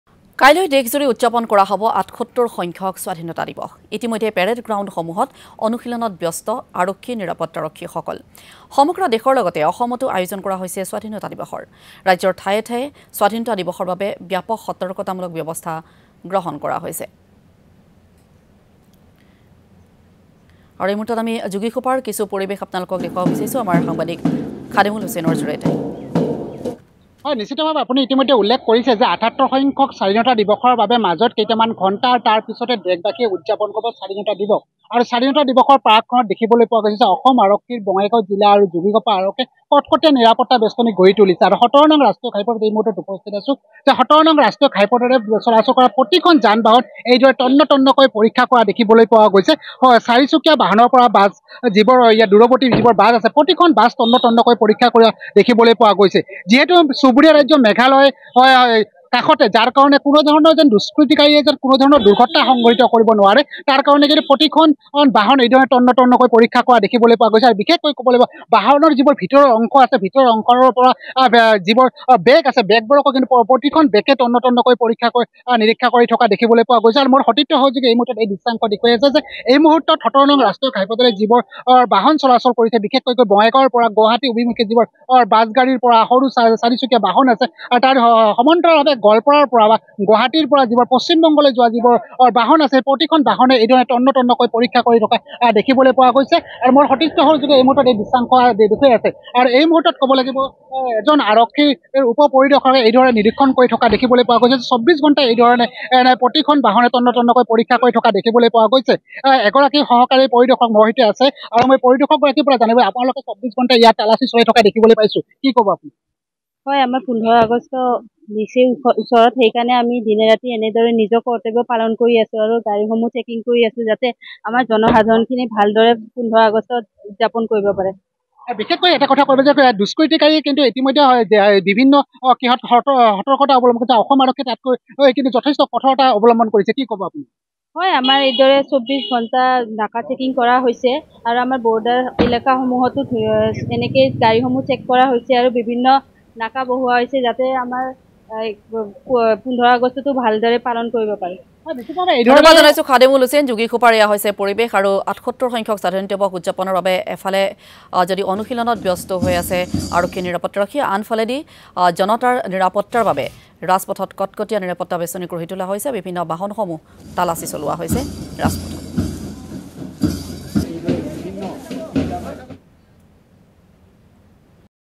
ৰাজ্যৰ ঠায়ে ঠায়ে স্বাধীনতা দিৱসৰ ব্যাপক প্ৰস্তুতি......
কাইলৈ দেশজুৰি উদযাপন কৰা হ'ব ৭৮ সংখ্যক স্বাধীনতা দিৱস। ৰাজ্যৰ ঠায়ে ঠায়ে স্বাধীনতা দিৱসৰ ব্যাপক প্ৰস্তুতি। স্বাধীনতা দিৱসৰ প্ৰাকক্ষণত কটকটীয়া নিৰাপত্তা ব্যৱস্থা গঢ়ি তুলিছে বঙাইগাঁও আৰু যোগীঘোপা আৰক্ষীয়ে। চাওক আমাৰ এক প্ৰতিবেদন।